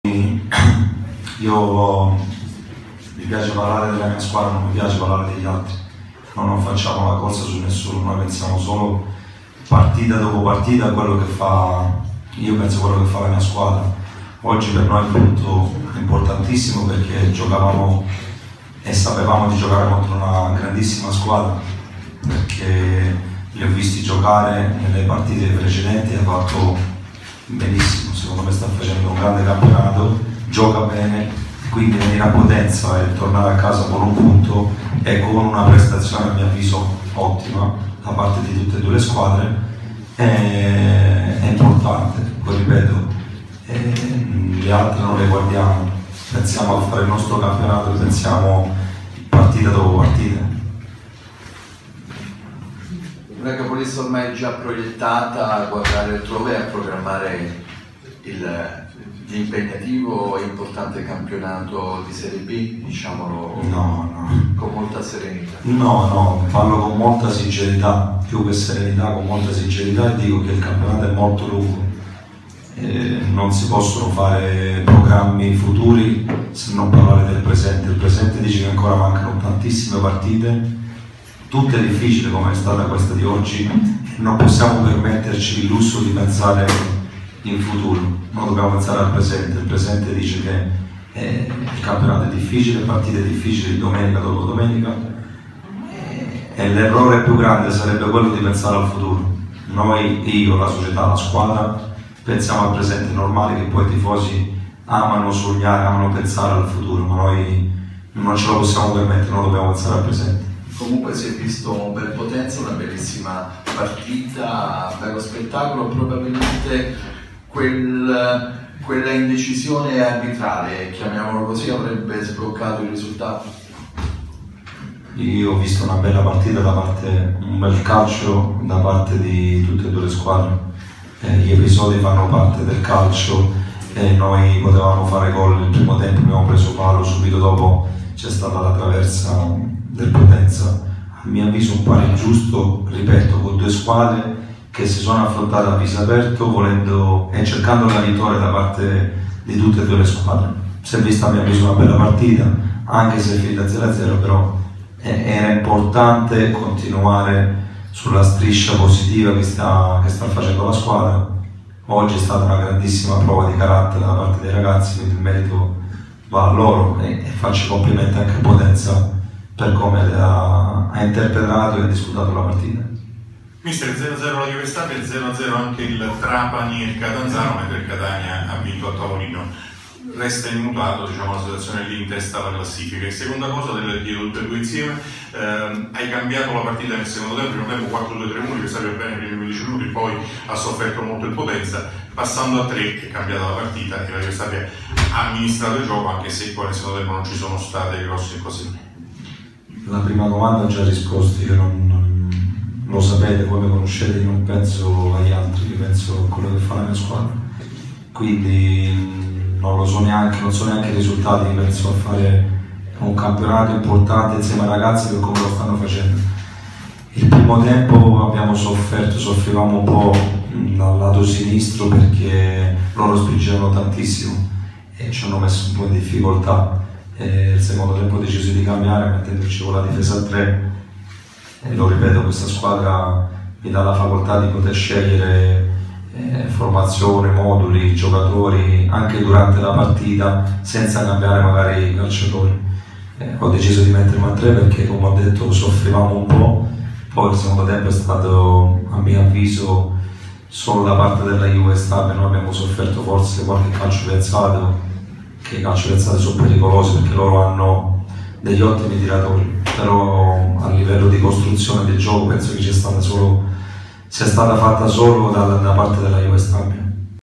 Caserta | Potenza-Juve Stabia 0-0
Le parole di mister Fabio Caserta al termine di Potenza-Juve Stabia 0-0